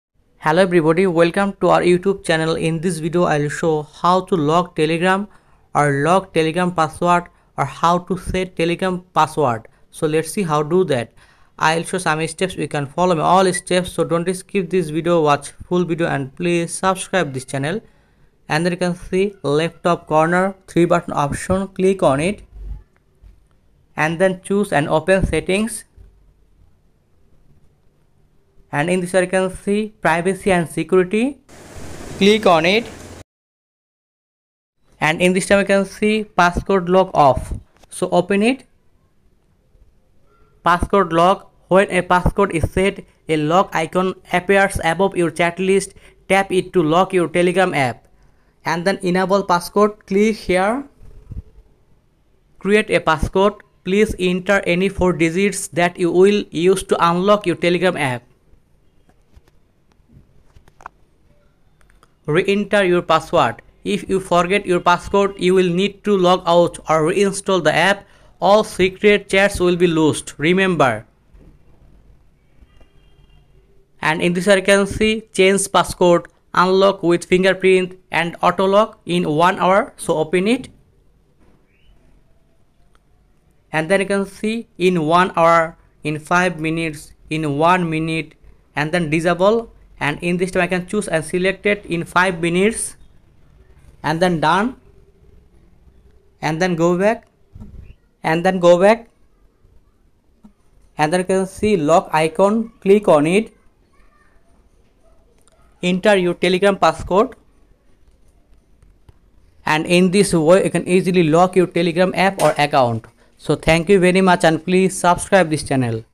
How to Lock Telegram Chats | Telegram Chat Lock
Hi Everyone,
In This video I Will Show How to Lock Telegram Chats  | Telegram App Lock.
You can lock your Telegram chats using Passcode Lock or Fingerprint Lock (if supported). Here's How to Do it.
on Mobile (Android & ios)
1. Open Telegram and go to Settings (three lines in the top left → Settings).
2. Tap Privacy and Security.
3. Scroll down to Passcode Lock (or App Lock on some versions).
4. Tap Turn on Passcode and set a 4-digit PIN or Password (you can also use fingerprint or Face ID if supported).
5. Toggle Auto-Lock to set a time after which Telegram locks automatically.
6. To lock a chat manually, tap the lock icon at the top of the chat list.
Now, to access locked chats, you'll need to enter the passcode or use biometrics.
Let me know if you need more details!